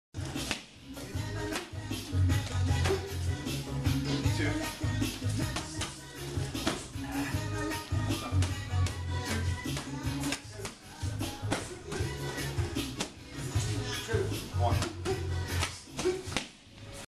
Ring Lords Vale Tudo Thai Pad Training
Jonathan D. And Bart D. (With Jonathon F. and Matthew P. in the background), just working some basic Thai boxing combos at Ring Lords, in Cary, NC.

Train hard!